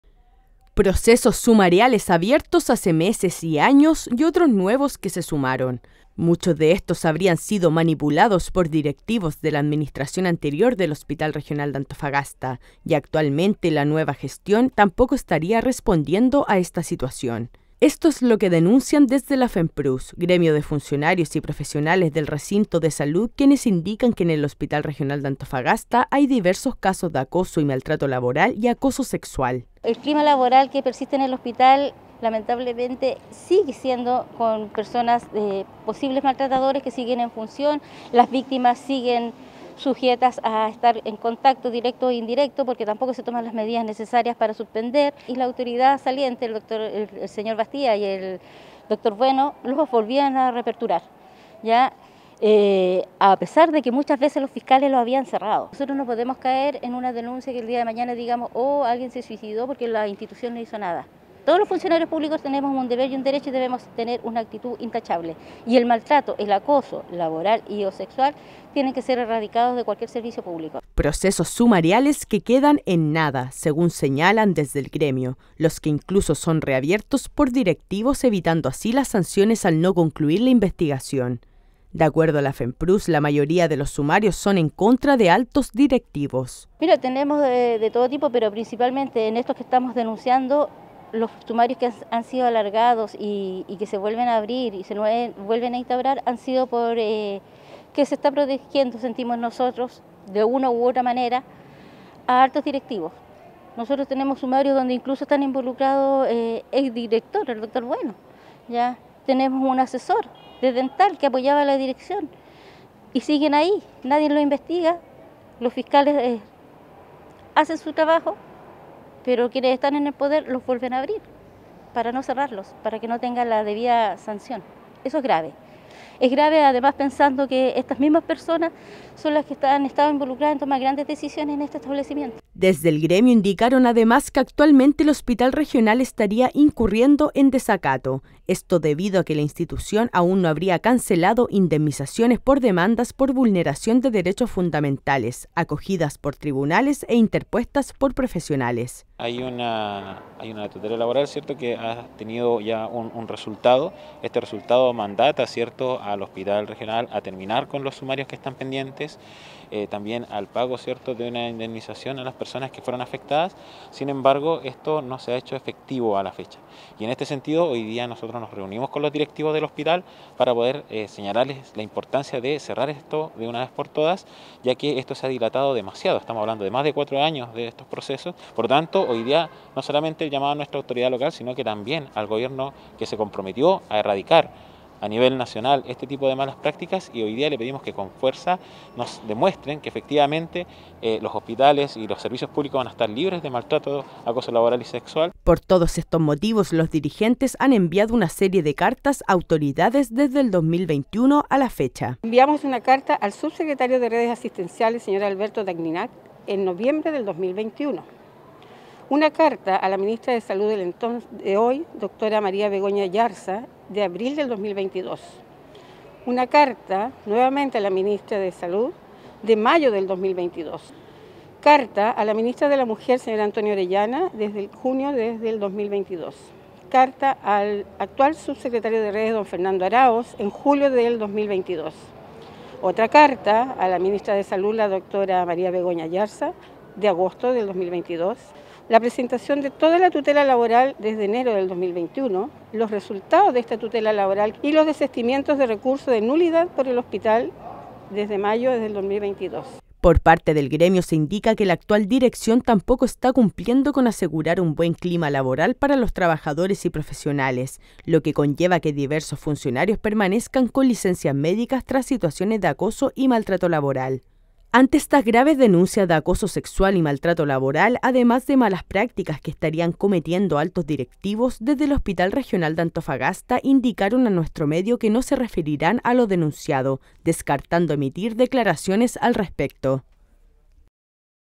Fenpruss denuncia nuevos casos de acoso laboral y sexual en Hospital Regional de Antofagasta
Una grave denuncia realizó la Fenpruss del Hospital Regional de Antofagasta. Maltrato y acoso laboral, acoso sexual, instrumentalización de procesos sumariales, son algunos de los hechos denunciados por dirigentes, quienes aseguran que directivos de administraciones anteriores, como de la actual gestión del recinto de salud, son responsables de mantener estas prácticas. Además, señalan que el Hospital estaría incurriendo en “desacato” tras no haber cancelado indemnizaciones por demandas por vulneración de derechos fundamentales…